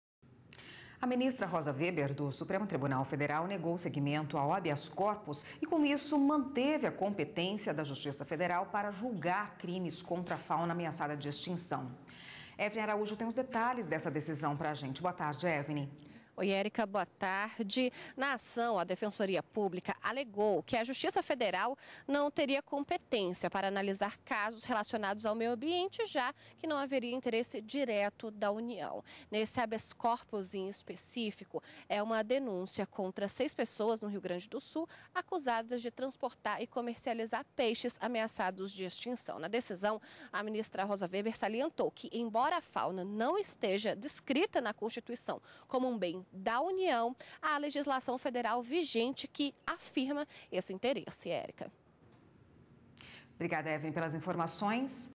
Crime contra a fauna (04/01/18)
A ministra Rosa Weber, do Supremo Tribunal Federal, negou seguimento a habeas corpus e com isso manteve a competência da Justiça Federal para julgar crime contra fauna ameaçada de extinção. Evinny Araújo tem os detalhes. Boa tarde Evinny.